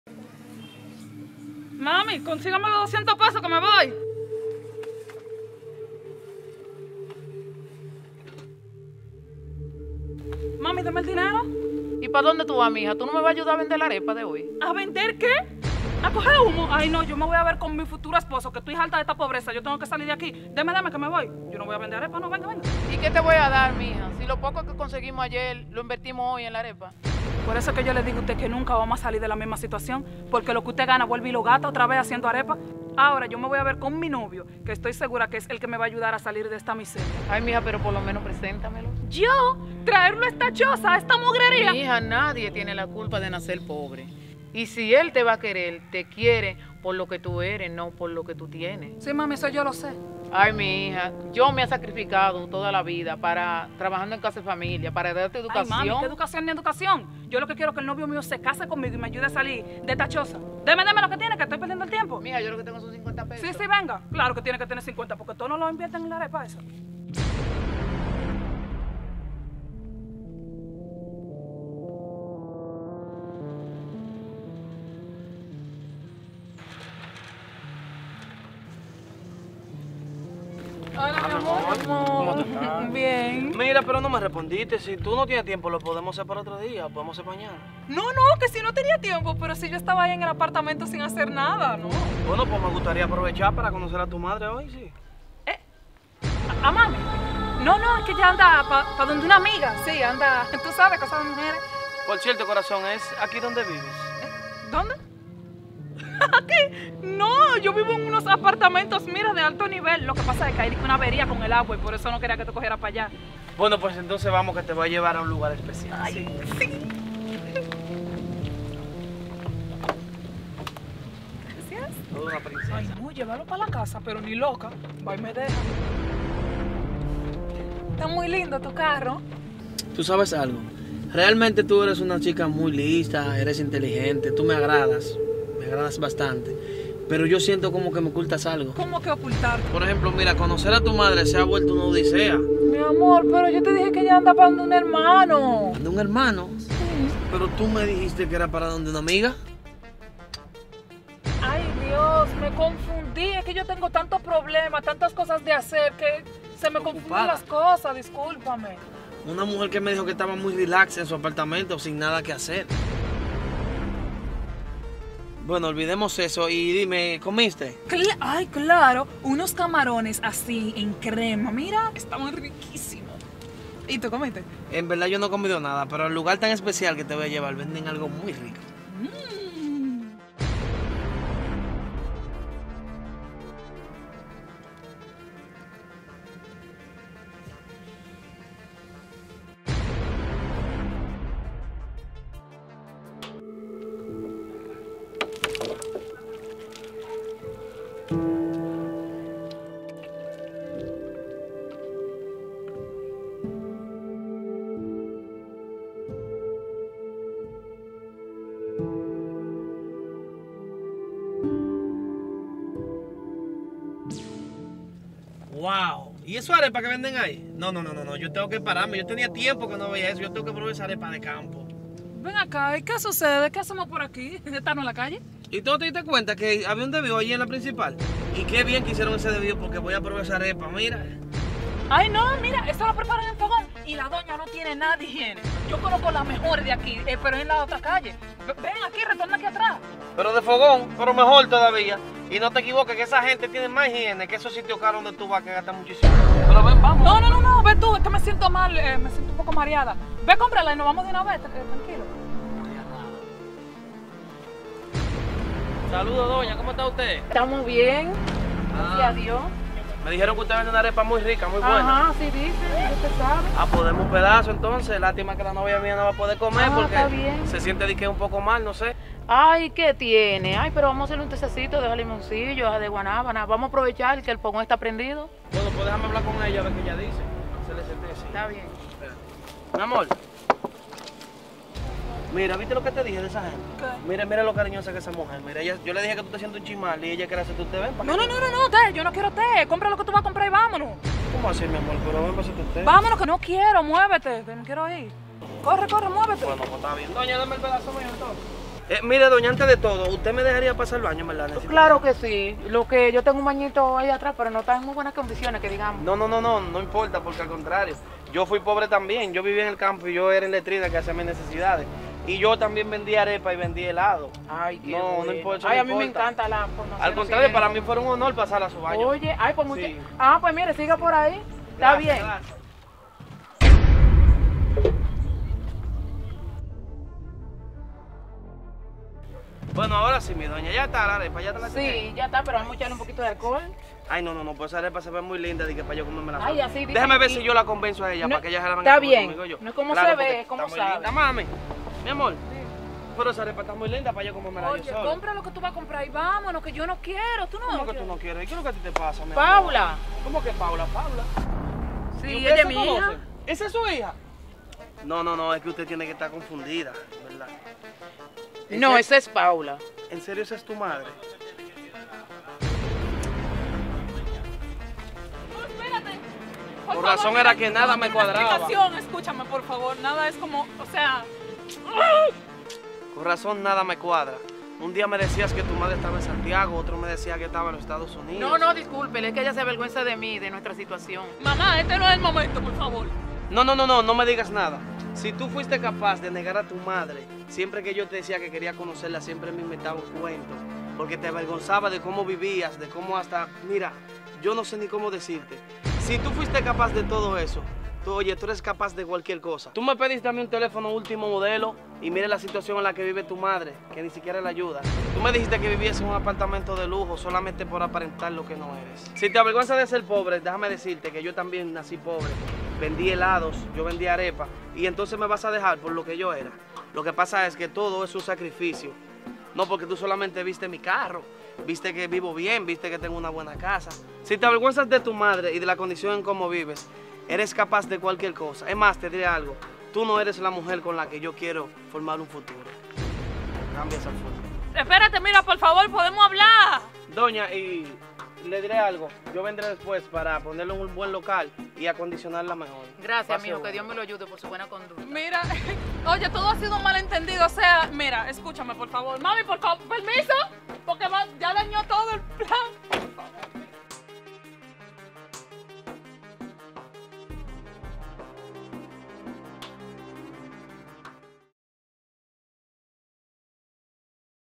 Rechazo ayudar a su MAMÁ a vender arepas, sin saber que esto pasaria - Nany Flow
Esta mujer recibe una gran lección de vida al sentirse avergonzada de su madre solo porque era pobre y ella no quería que los demás supieran la verdad...